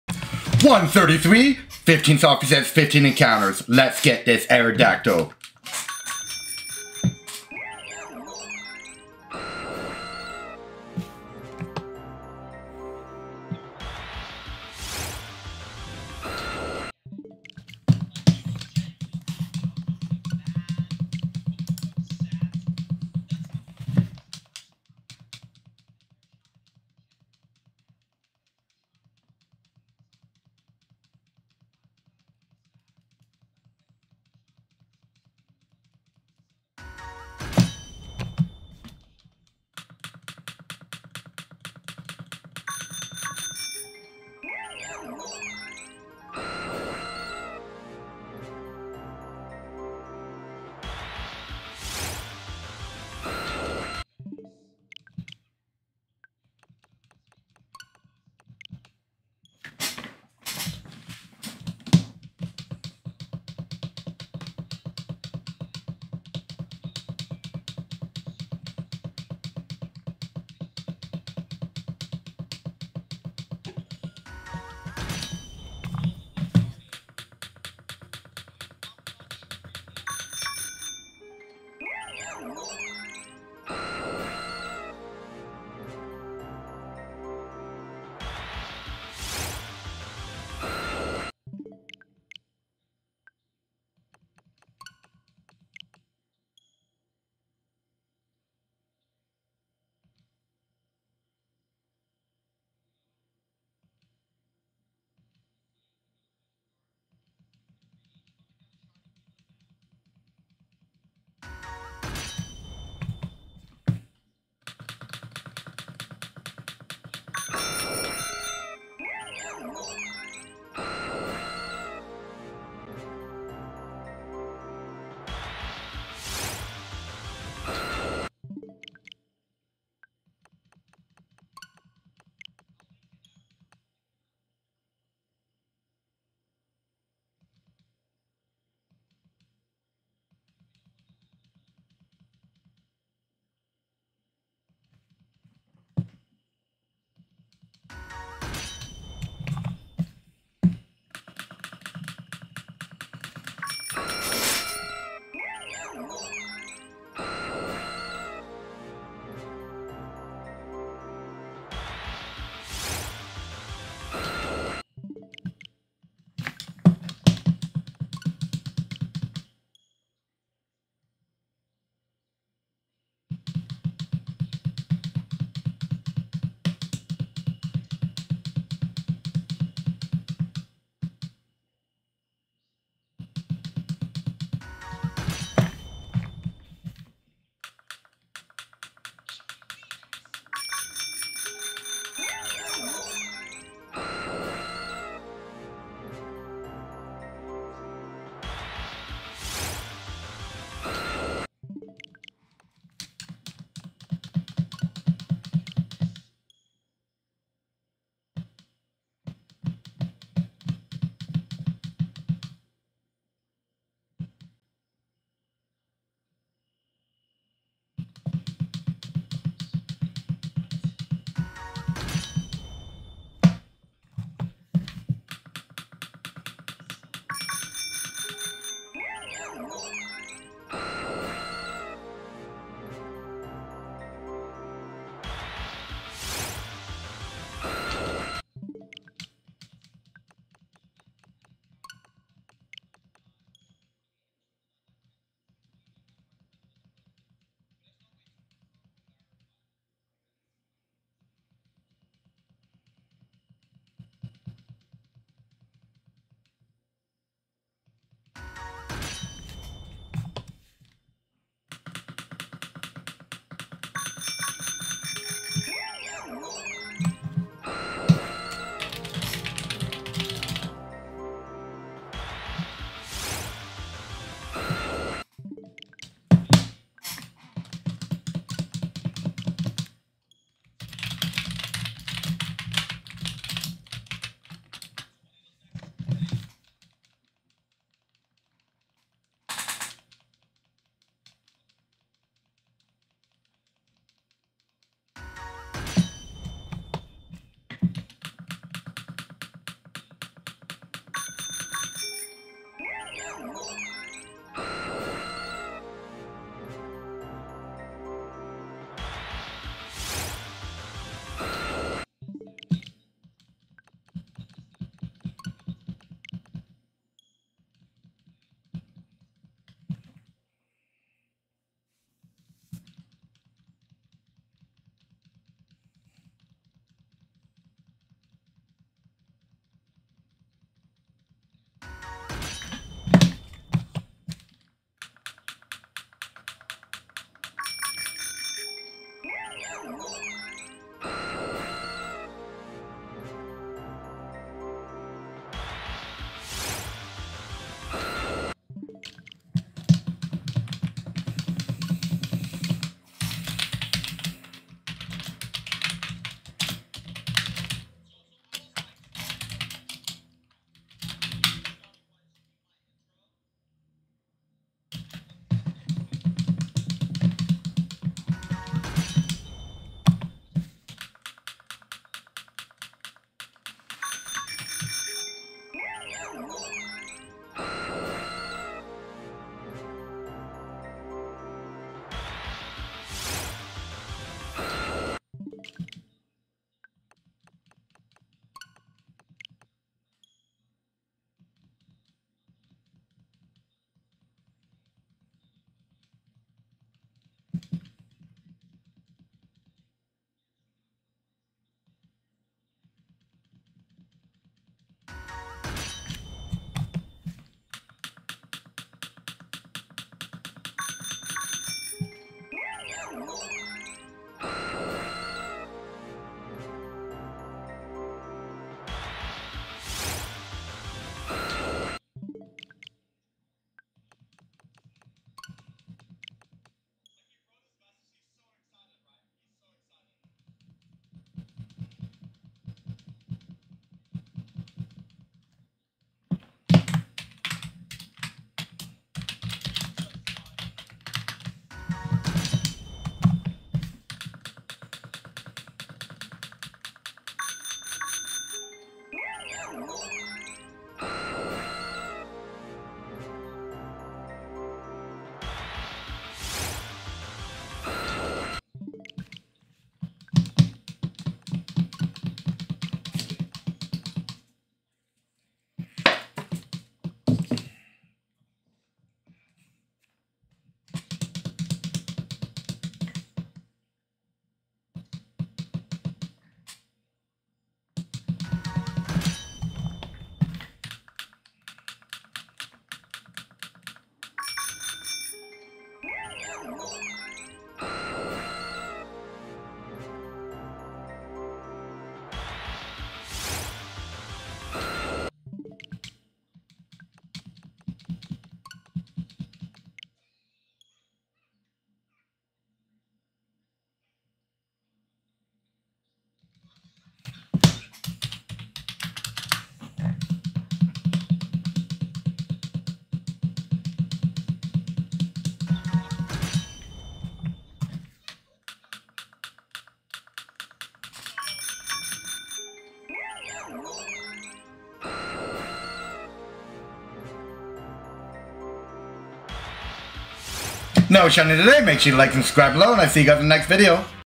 Day 133: 15 SOFT RESETS FOR A SHINY AERODACTYL!
We’ll be doing 15 srs every day to see if we can pull the EPIC shiny hunt were looking for. The one we are doing currently is SHINY AERODACTYL. Hope you enjoy the short and you never know what day we could pull this shiny xD!!! Enjoy. Day 133: 15 SOFT RESETS FOR A SHINY AERODACTYL!